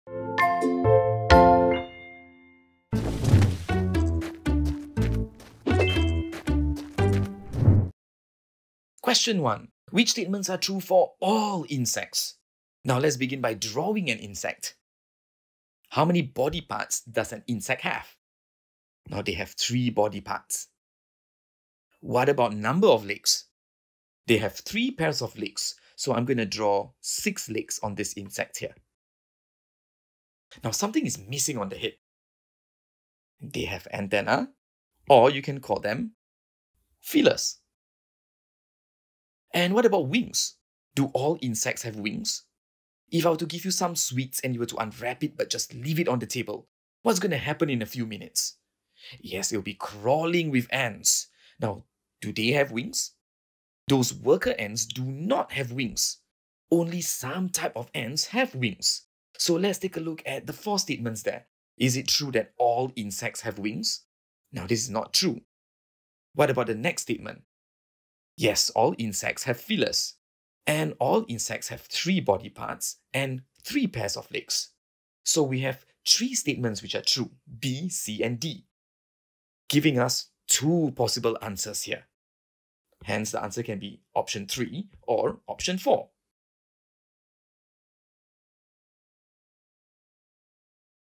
EXAMINATION PAPER SERIES | 2019 SNGS P5 SA2 | Question 1 | Animal Classification 🐜
In this Examination Paper Series, Mr. Firhad will be analysing Question 1 from the 2019 CHIJ St. Nicholas Girls' School (SNGS) P5 SA2 Science Examination paper.

Watch closely as he shares his approach in tackling this question on Animal Classification!